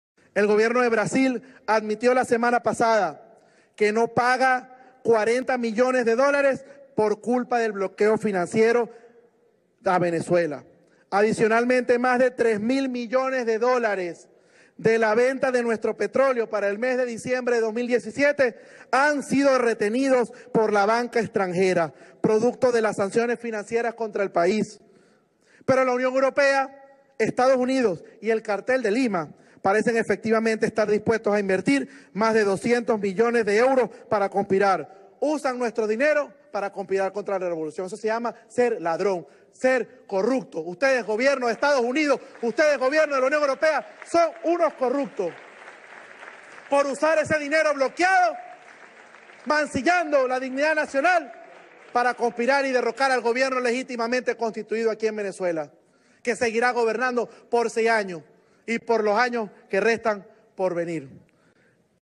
NICOLASITO MADURO EN LA ANC|| “LA UNIÓN EUROPEA, EEUU Y EL CARTEL DE LIMA SON UNOS CORRUPTOS”
El constituyentista Nicolás Maduro Guerra, hijo de Nicolás Maduro, aseguró este martes que La Unión Europea, EEUU y el Cartel de Lima “son unos corruptos”. Además, afirmó que utilizan el dinero bloqueado por las sanciones impuestas al gobierno nacional para “conspirar contra la revolución bolivariana”.
“La Unión Europea, EEUU y el Cartel de Lima son unos corruptos. Usan nuestro dinero bloqueado para conspirar contra la revolución. Eso se llama ser ladrón, ser corruptos. Ustedes, Gobiernos aliados de Estados Unidos y la Unión Europea son corruptos”, dijo durante la sesión de la Asamblea Nacional Constituyente (ANC).